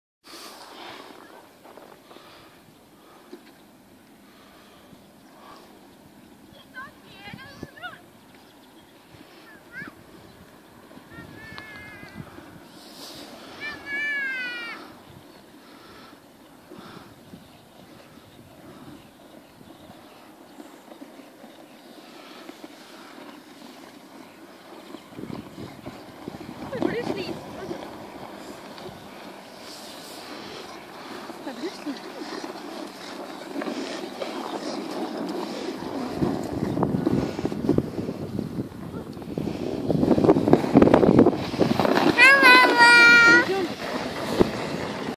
Bulldog acting as sledge dog
Optimi Amici Donald Duck is pulling our daughter, Aurora